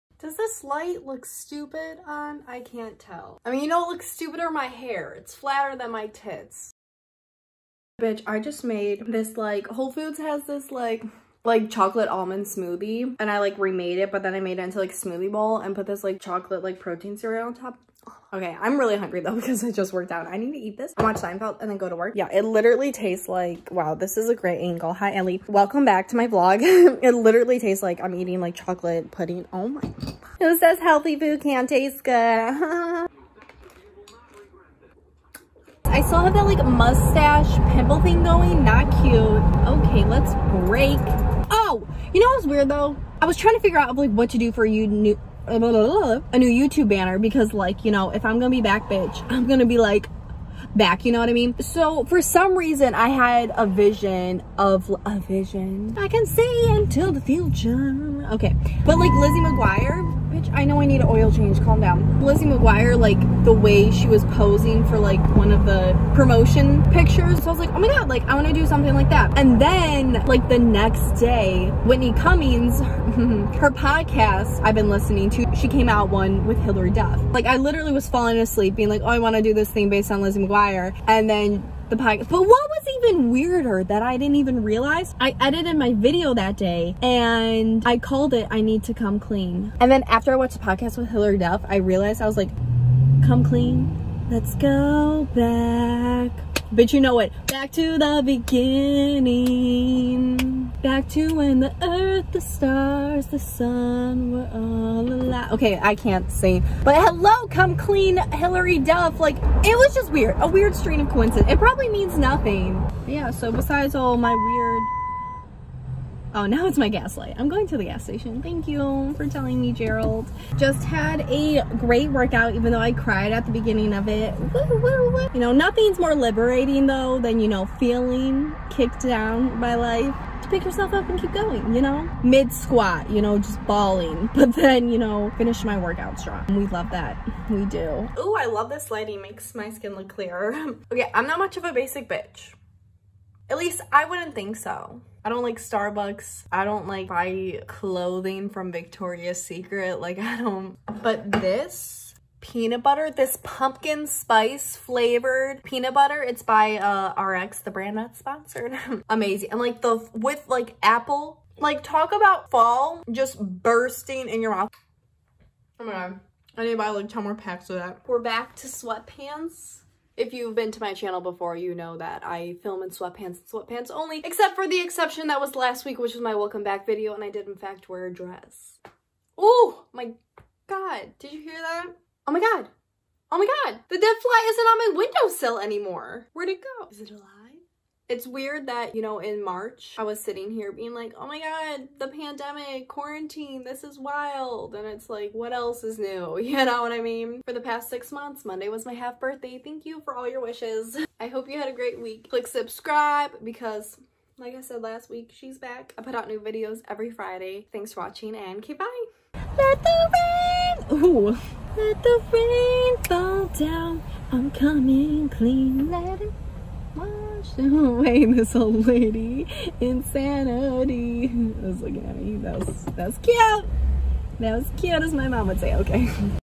How Lizzie McGuire Changed My Life | Vlogs About Nothing
"Why not?" - Hilary Duff
This week is about how Lizzie McGuire changed my life...as in inspired my Youtube channel update hehe. But it also actually changed my life, like Hilary Duff? Still one of my biggest inspirations.

New Vlogs About Nothing out every Friday!